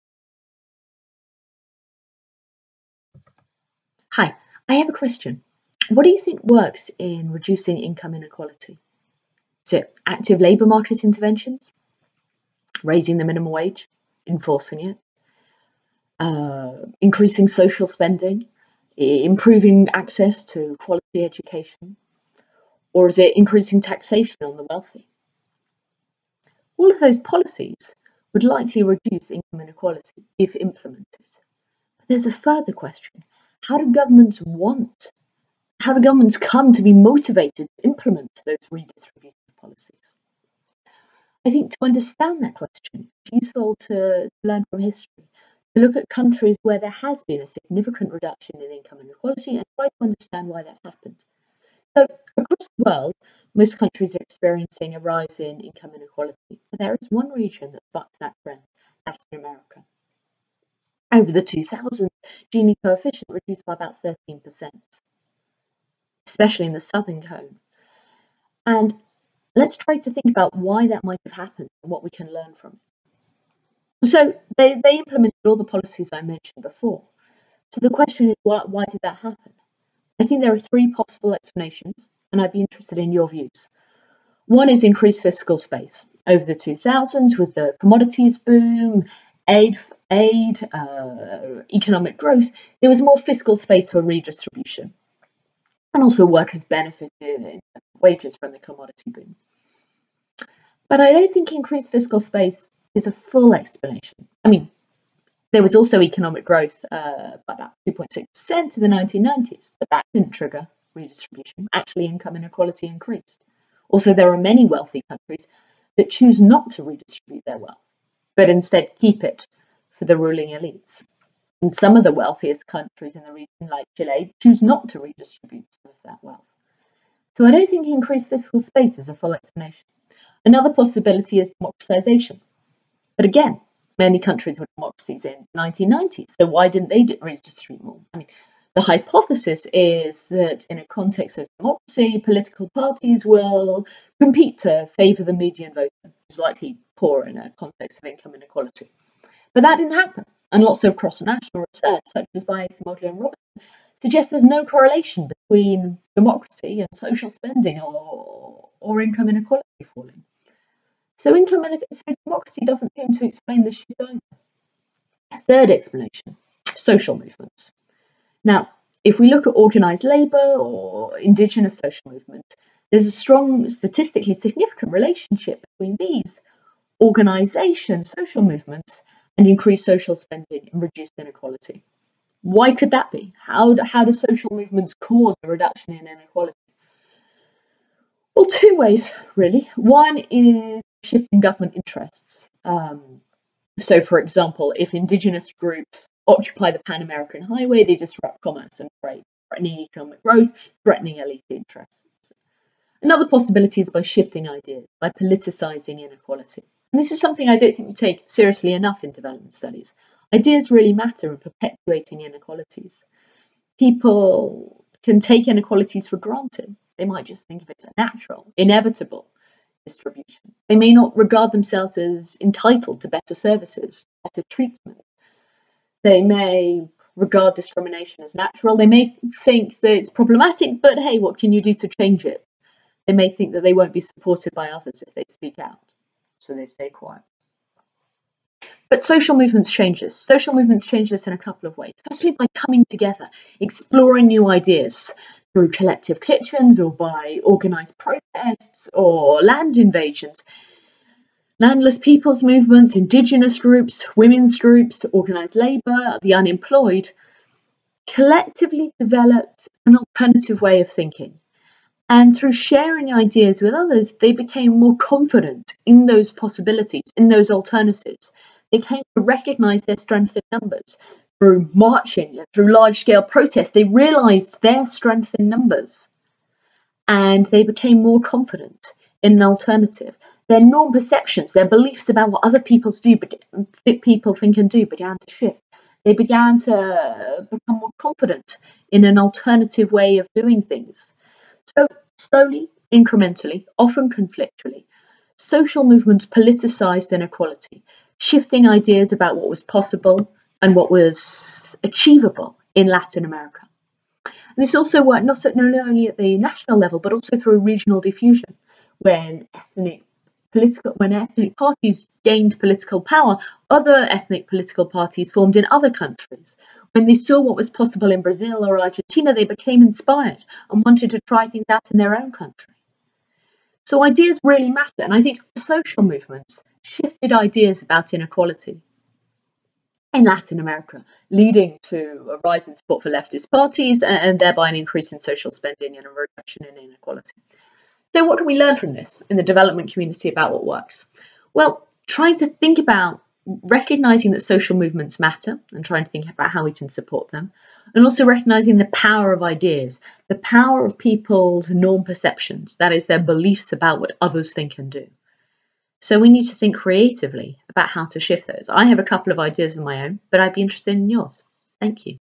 What works in reducing income inequality?
About the speaker:
Dr. Alice Evans is a lecturer at King's College London and an associate at the Center for International Development at Harvard University, with previous appointments at Cambridge and the LSE. She is writing a book on "The Global Politics of Decent Work".

Through comparative research on strengthening corporate accountability, Dr. Evans explores how to resolve global collective action problems and improve workers' rights. She has published on the causes of falling inequality in Latin America; social movements; rising support for gender equality; cities as catalysts of social change; and the politics of maternal mortality.

Follow Dr. Evans on Twitter: @_alice_evans